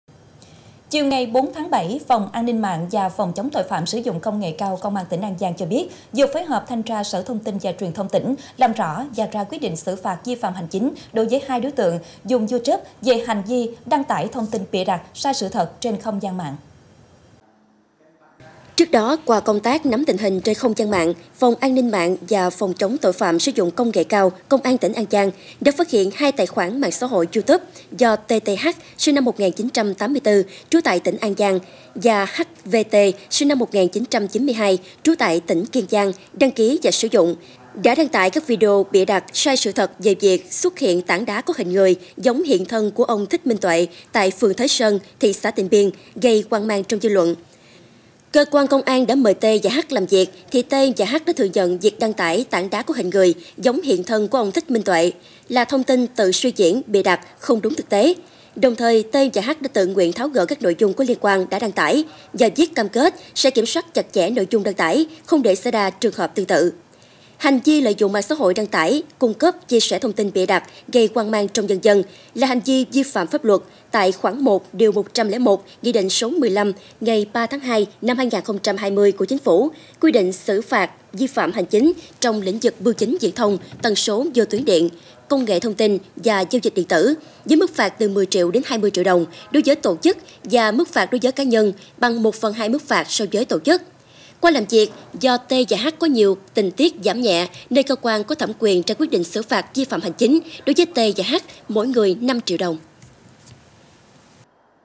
Xử phạt 02 người đăng tải thông tin bịa đặt – sai sự thật lên không gian mạng xã hội
Trước đó, qua công tác nắm tình hình trên không gian mạng, Phòng An ninh mạng và phòng, chống tội phạm sử dụng Công nghệ cao, Công an tỉnh An Giang đã phát hiện 2 tài khoản mạng xã hội YouTube do T.T.H sinh 1984, trú tại tỉnh An Giang và H.V.T sinh 1992, trú tại tỉnh Kiên Giang đăng ký và sử dụng, đã đăng tải các video bịa đặt, sai sự thật về việc xuất hiện tảng đá có hình người, giống hiện thân của ông Thích Minh Tuệ tại phường Thới Sơn, thị xã Tịnh Biên, gây hoang mang trong dư luận.
Cơ quan công an đã mời T và H làm việc thì T và H đã thừa nhận việc đăng tải "tảng đá có hình người, giống hiện thân của ông Thích Minh Tuệ" là thông tin tự suy diễn, bịa đặt, không đúng thực tế. Đồng thời, T và H đã tự nguyện tháo gỡ các nội dung có liên quan đã đăng tải và viết cam kết sẽ kiểm soát chặt chẽ nội dung đăng tải, không để xảy ra trường hợp tương tự.
Hành vi lợi dụng mạng xã hội đăng tải, cung cấp, chia sẻ thông tin bịa đặt, gây hoang mang trong nhân dân… là hành vi vi phạm pháp luật, tại khoản 1 Điều 101 Nghị định số 15 ngày 3/2/2020 của Chính Phủ "Quy định xử phạt vi phạm hành chính trong lĩnh vực bưu chính, viễn thông, tần số vô tuyến điện, công nghệ thông tin và giao dịch điện tử" với mức phạt từ 10 triệu đến 20 triệu đồng đối với tổ chức và mức phạt đối với cá nhân bằng 1/2 mức phạt so với tổ chức.
Qua làm việc, do T và H có nhiều tình tiết giảm nhẹ nên cơ quan có thẩm quyền ra quyết định xử phạt vi phạm hành chính đối với T và H mỗi người 5 triệu đồng./.